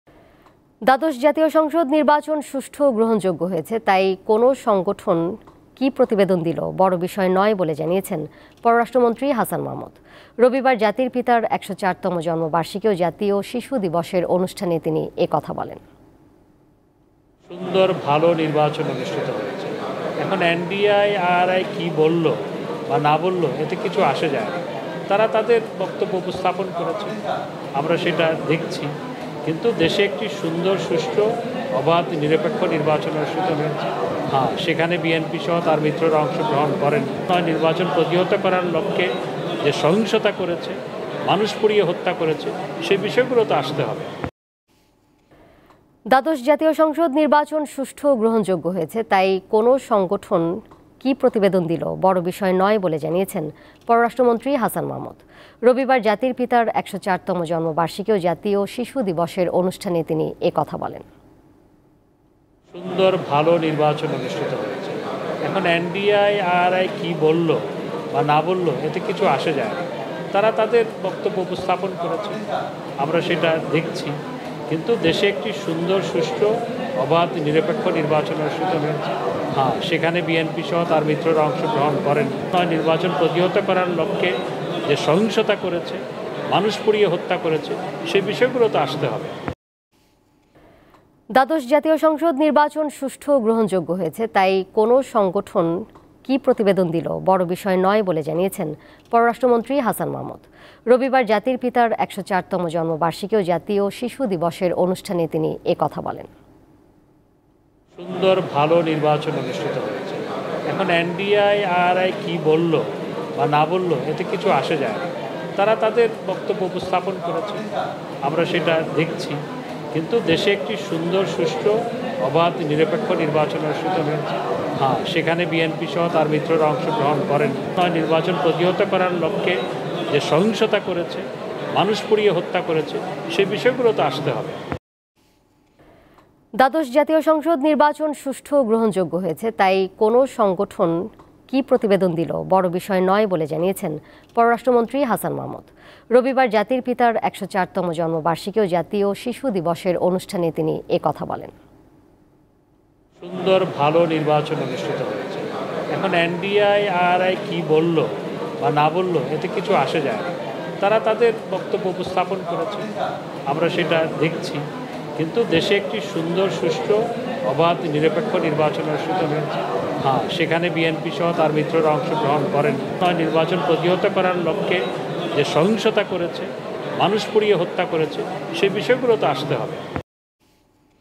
নির্বাচন নিয়ে এনডিআই’র প্রতিবেদনে কিছু আসে যায় না: পররাষ্ট্রমন্ত্রী | Foreign Minister | Election
নির্বাচন নিয়ে এনডিআই’র প্রতিবেদনে কিছু আসে যায় না: পররাষ্ট্রমন্ত্রী

Address:
Deepto TV
7/A/GA Tejgaon Industrial Area,